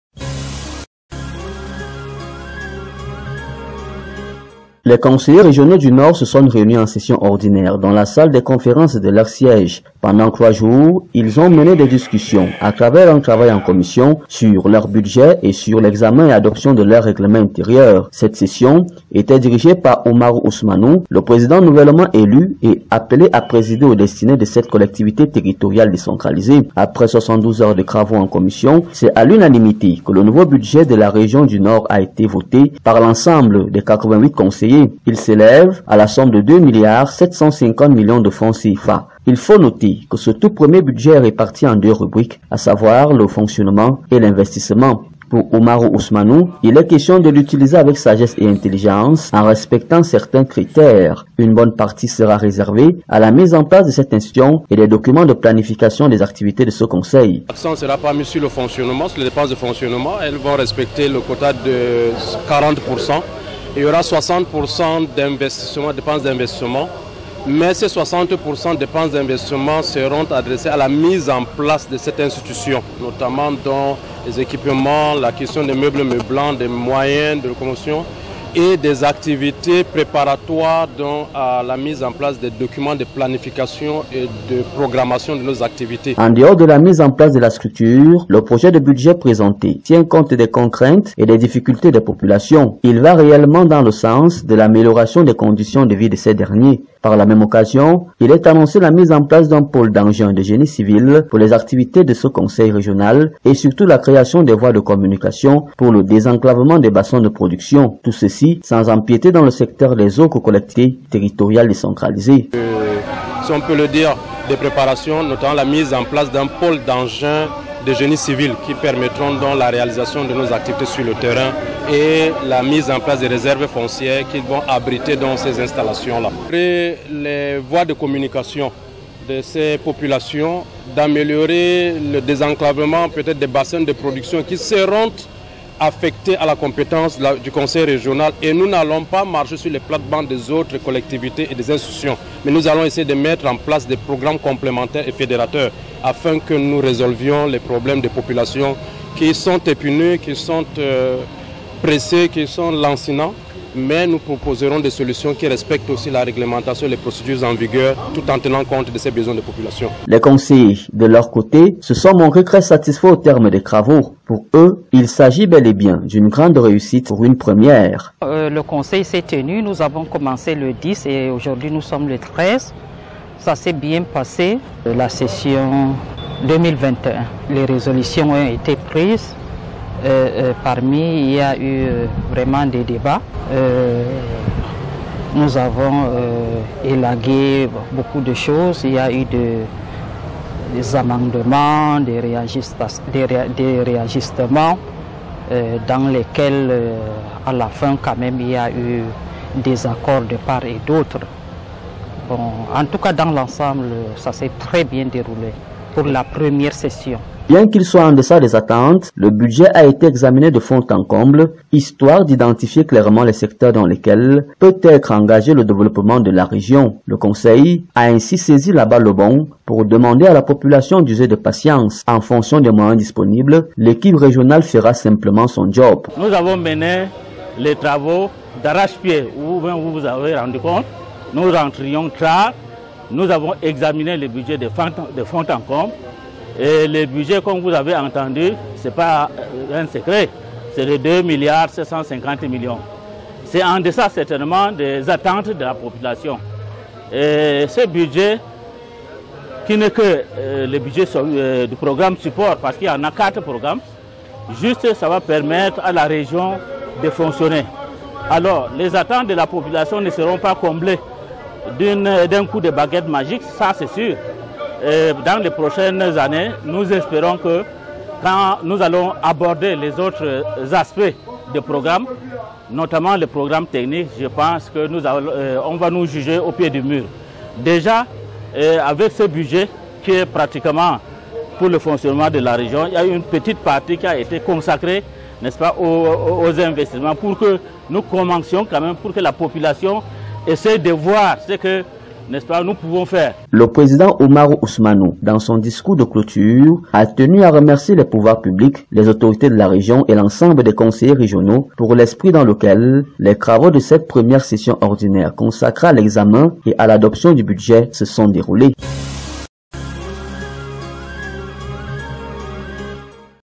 Nord-Cameroun. Le conseil région a son tout 1er budget. 2 milliard 750 millions de FCFA
Après 72 heures des travaux en commissions, c’est à l’unanimité que le nouveau budget de la région du Nord a été voté par l’ensemble des 88 conseillers. Il s’élève à la somme de 2 milliard 750 millions de FCFA.